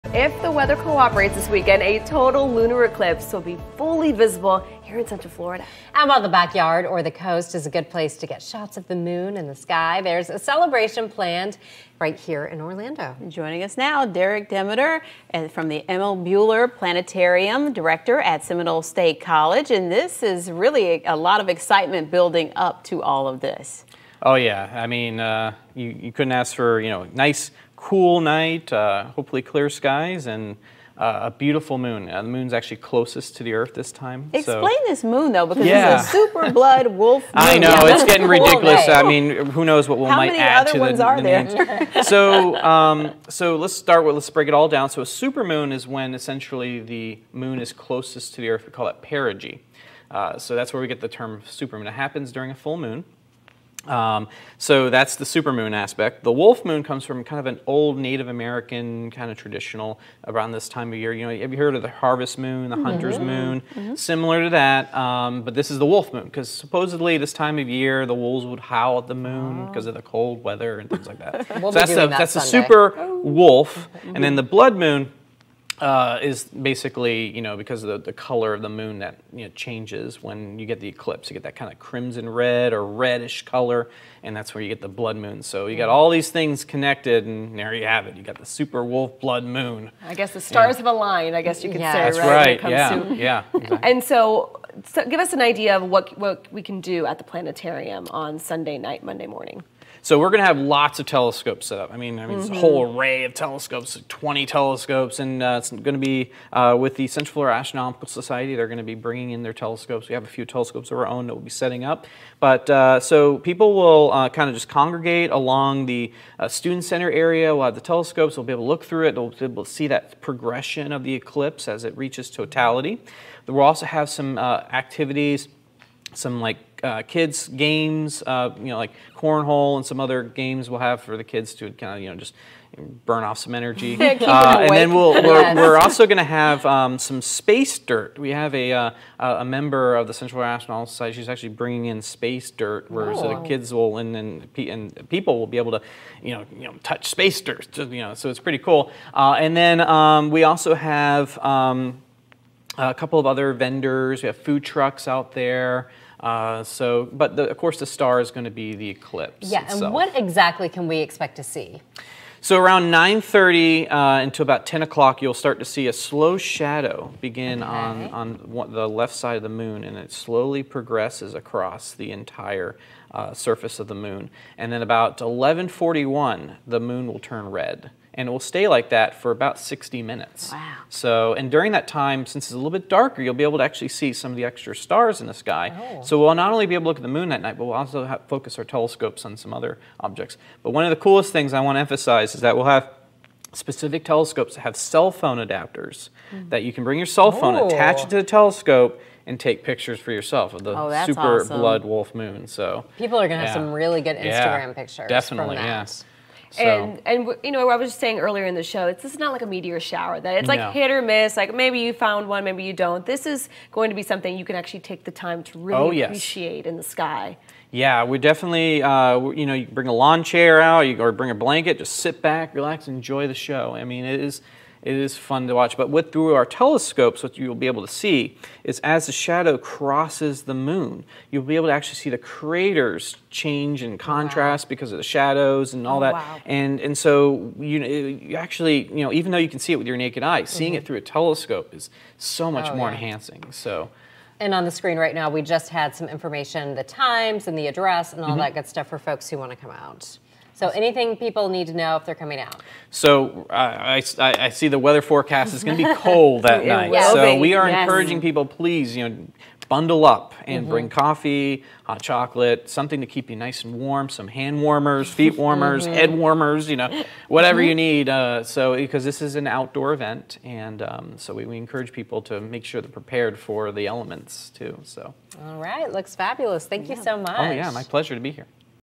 Total lunar eclipse to be fully visible this weekend
Seminole State Planetarium Director Derek Demeter explains the super wolf blood moon crossover and planetarium event